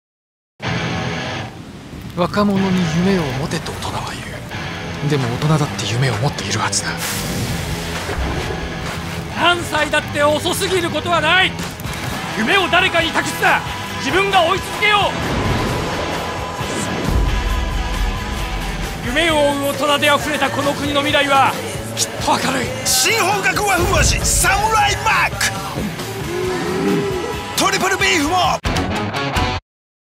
堺雅人出演／マクドナルド新CM「サムライマック『夢を追う、サムライたちへ。』」篇
俳優の堺雅人（48歳）が、マクドナルド「サムライマック」テレビCMシリーズの新作「サムライマック『夢を追う、サムライたちへ。』」篇に出演。4月5日より放映を開始する。